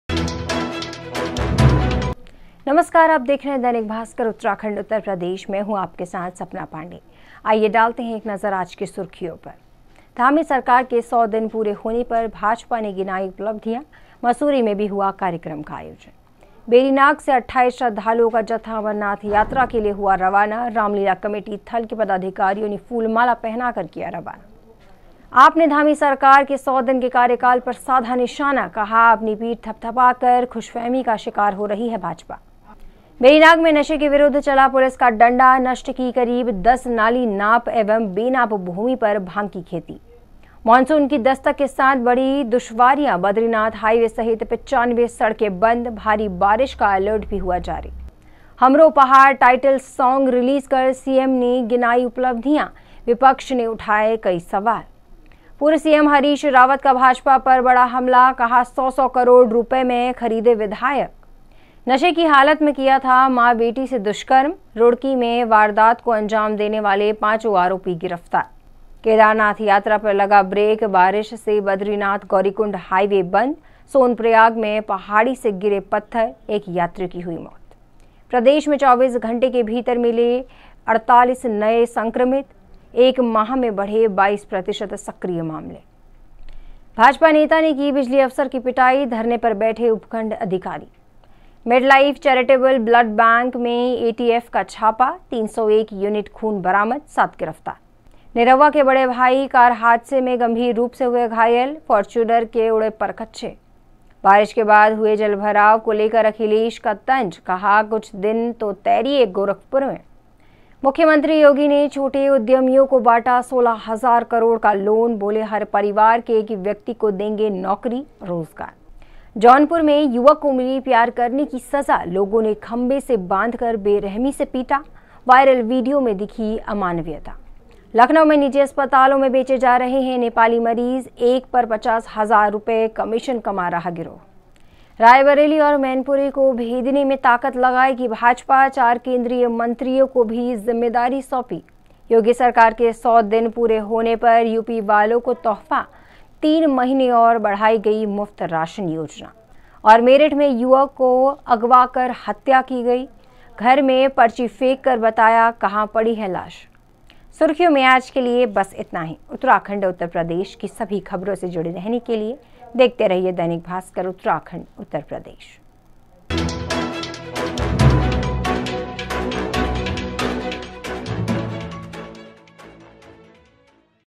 Bhaskar News Bulletin : Friday July 01 2022 Headline 20 news
जुलाई ०१, २०२१ के समाचार इस प्रकार है ...

Dainik Bhaskar Uttarakhand and UP Daily News Bulletin for July 01 2022

Presenter                                  :         Sapna Pandey
Editor                                         :         Mansvi Sharma